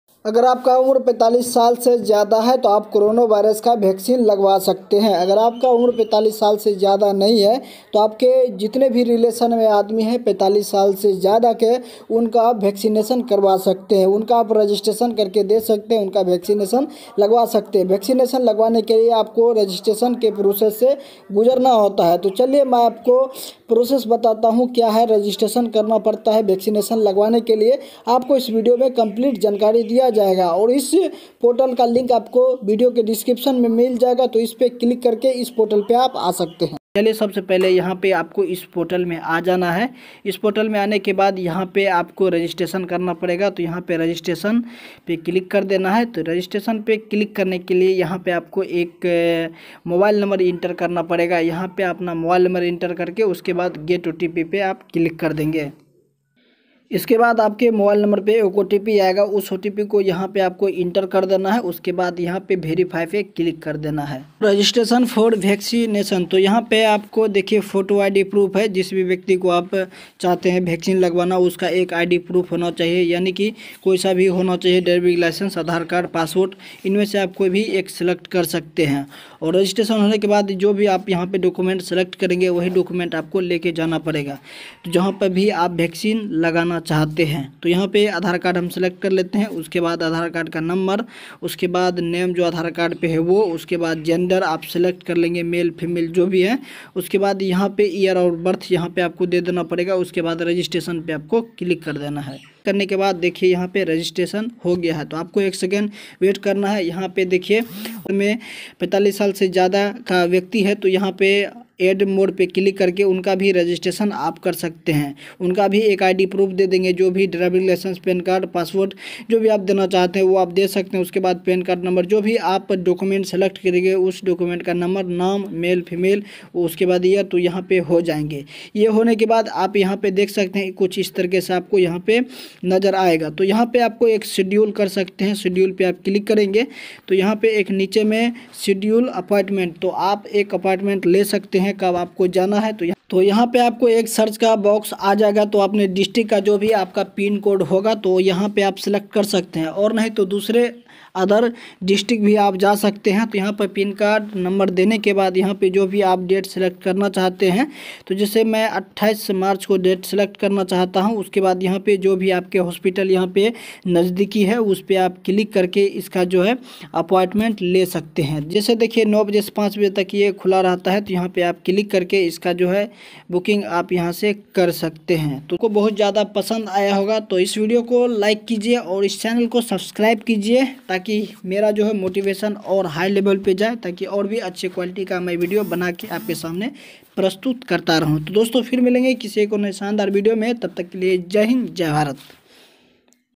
Vacation लगाने के लिए रीजेस्ट्रेशन करे ! corona vacation registration kaise kare ! covid 19 vax
Hello dosto ajj ki es video pe mai aplogo  ko bataunga ki Vacation लगाने के लिए रीजेस्ट्रेशन करे   corona vacation registration kaise kare ! covid 19 vax

● Green screen

● gorilla tripod

● vivo Z 1 pro(mobile)